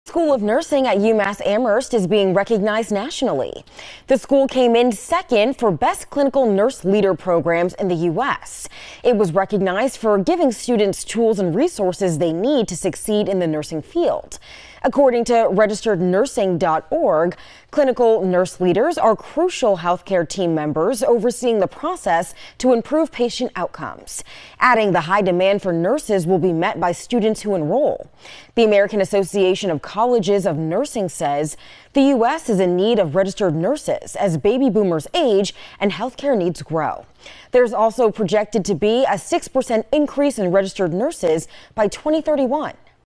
UMass Amherst Clinical Nurse Leader Program recognized as one of best in U.S.
UMass Amherst was named the 2nd Best Clinical Nurse Leader (CNL) Program in the nation by RegisteredNursing.org.